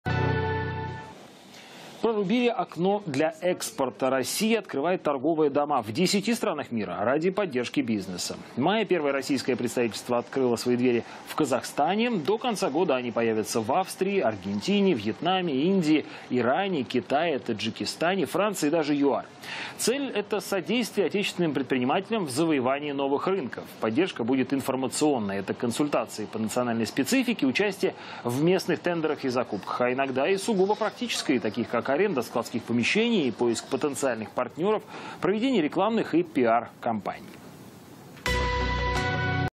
Россия построит торговые дома в десяти странах мира
Представительства помогут российским экспортерам продвигать товары